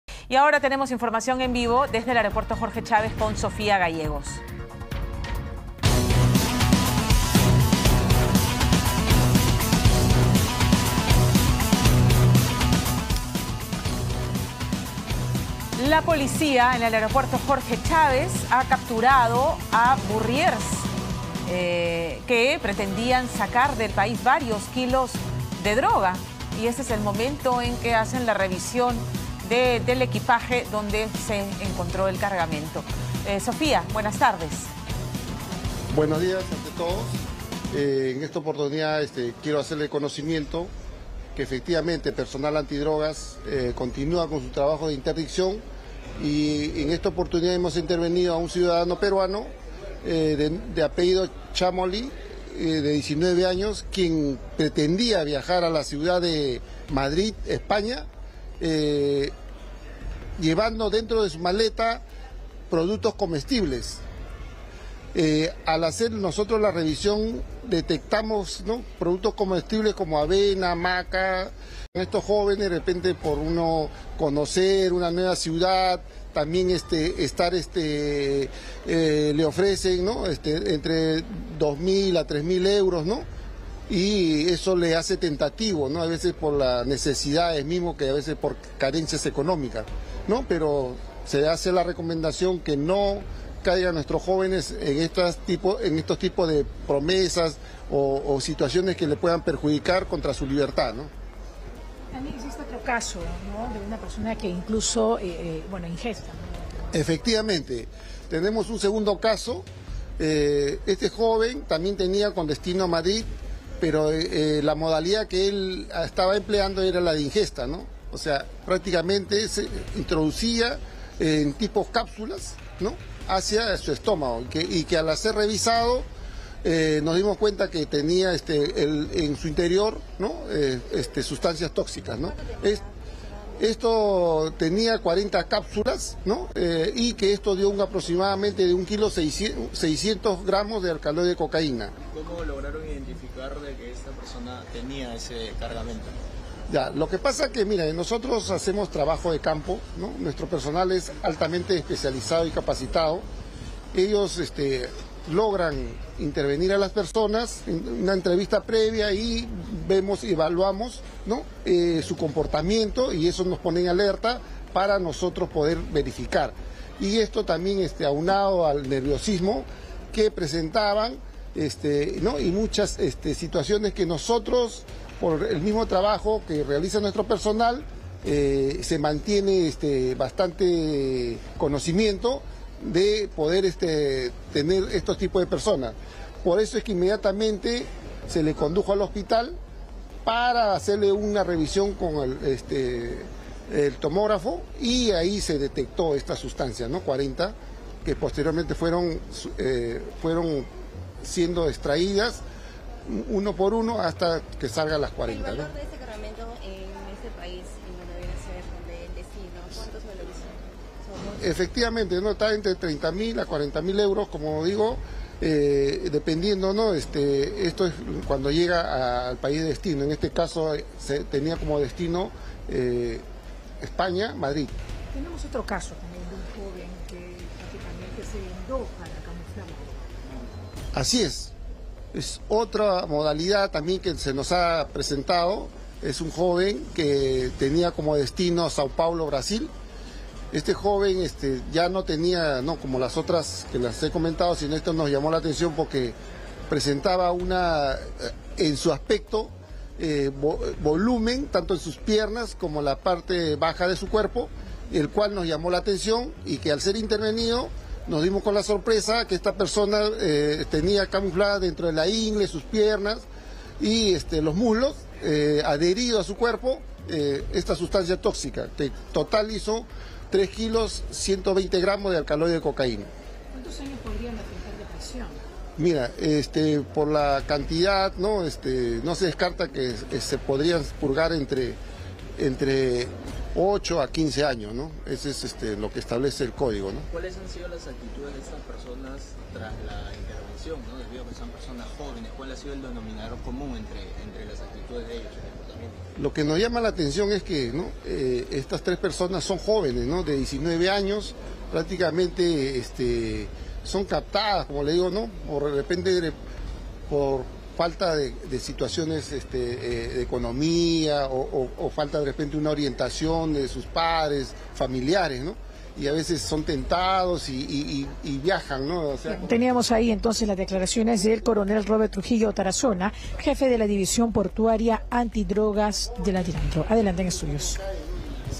Aeropuerto Jorge Chávez: Capturan a 'burriers' que intentaban pasar drogas
La Policía logró la captura de jóvenes que planeaban transportar sustancias ilícitas al extranjero.

Las autoridades señalaron que el joven de 19 años capturado tenía por destino la ciudad de Madrid. Se realizó un llamado a los ciudadanos para tomar conciencia sobre las falsas oportunidades ofrecidas a los jóvenes que terminan por arruinarles la vida e incluso, llevarlos a una penitenciaría.

En otro caso, otro joven habría introducido cápsulas en su estómago con el fin de transportarlas hasta Madrid. Se determinó que las 40 cápsulas halladas en el organismo del joven superaban el kilo de alcaloide de cocaína. El joven fue conducido a un centro médico para extraer las cápsulas de su cuerpo.